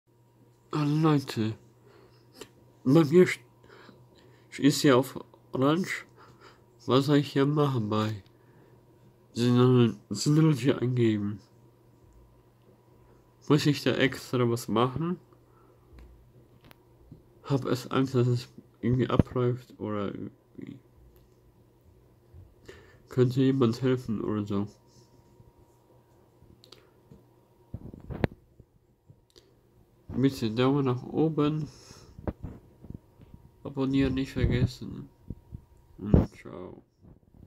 Bei mir ist Orange auf synology bei Zertifikaten
Ich brauche Hilfe ich weiß nicht was soll ich machen bei mir ist Orange